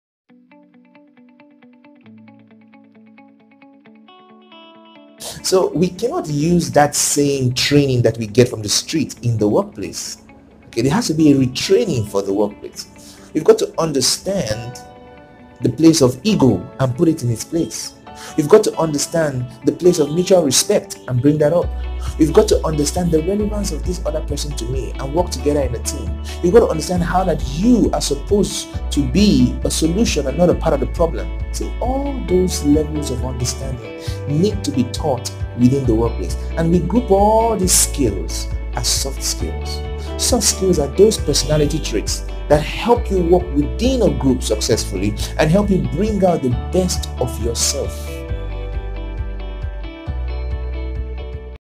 Why Soft Skills Training is Important
We cannot take the same training from the street into the workplace. There needs to be a retraining in key soft skills for every single team member.